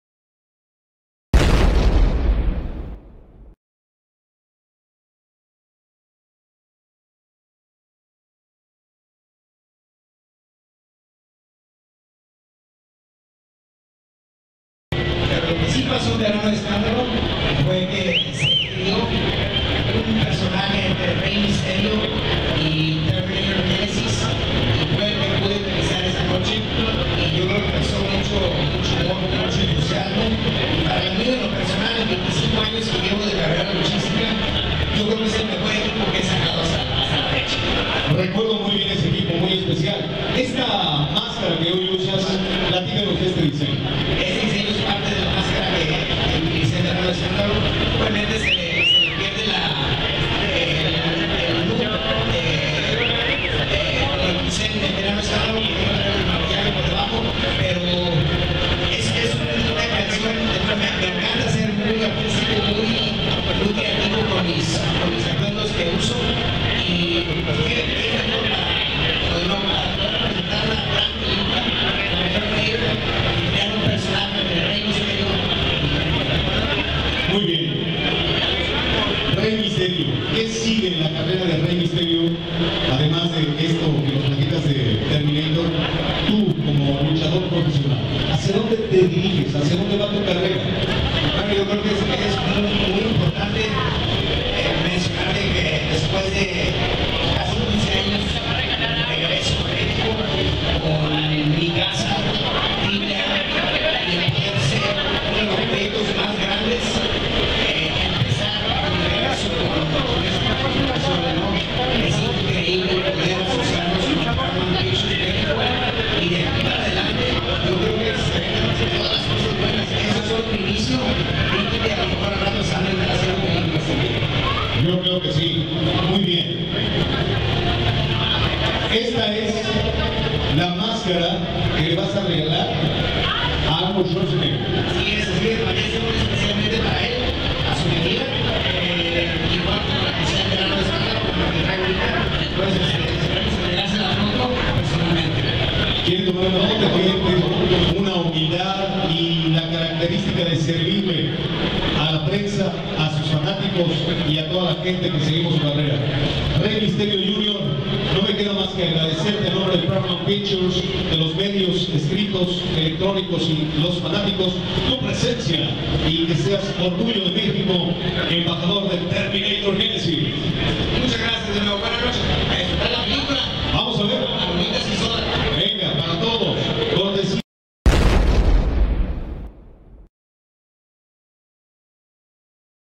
Rey Mysterio Jr. en la premier de Terminator Génesis
Alfombra roja de la premier de la película Terminator Génesis con la presencia del luchador de AAA Rey Mysterio Jr. 1 de julio 2015.